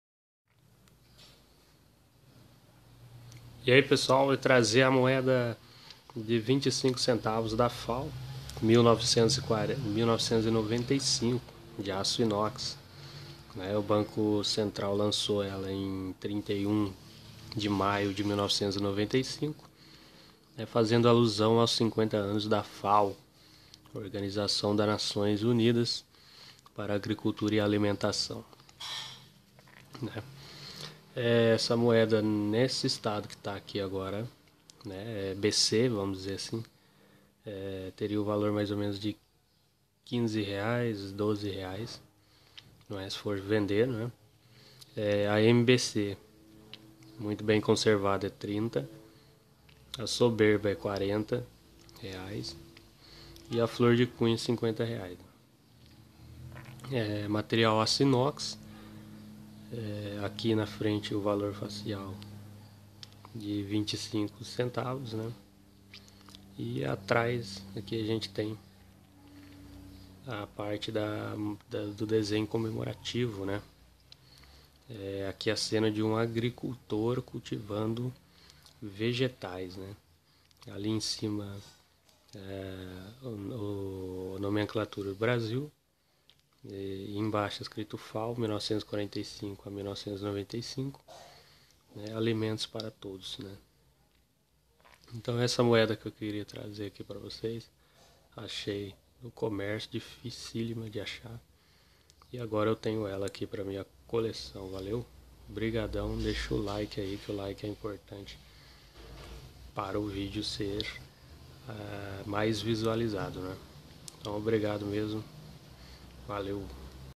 25 centavos FAO 1995
hj pessoal trago um video sobre a moeda que achei no comercio moeda comemorativa dos 50 ano das organizações das nações unidas para agricultura e alimentação